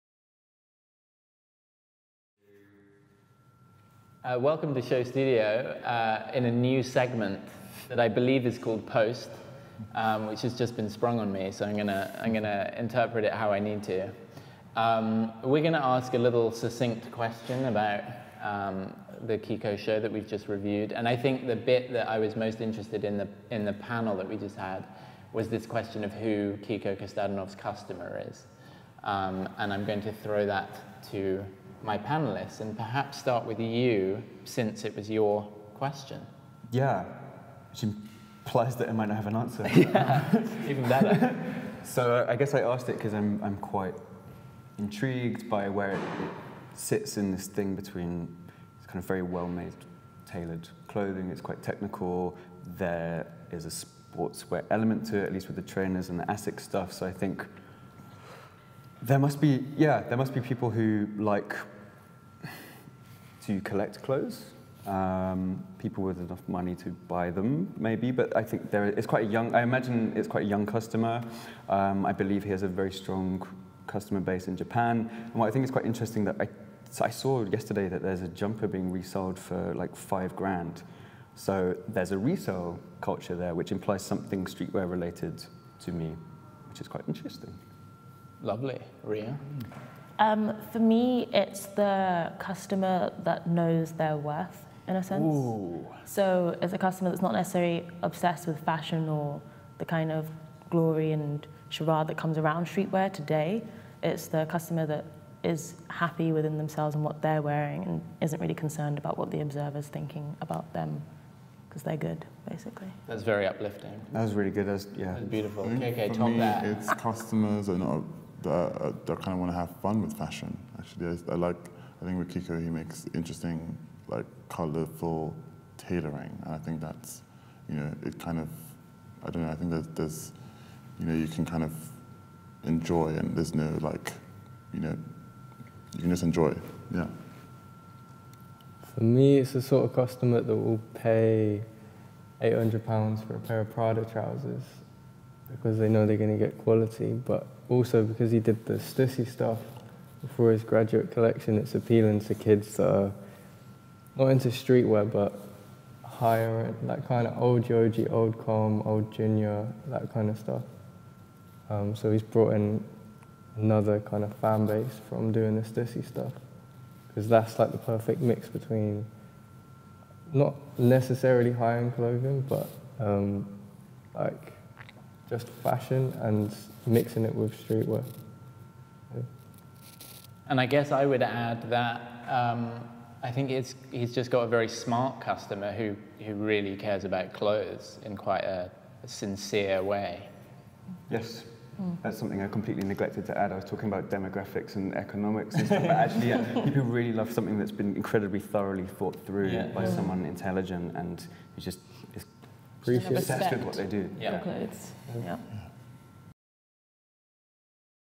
Who is the Kiko Kostadinov customer? - Post: Kiko Kostadinov A/W 19
KK Obi, Daniel Pacitti, Rhea Dillon and Eliot Haworth join guest chair Finn Mactaggart in Post to give their succinct thoughts on who the Kiko Kostadinov customer is.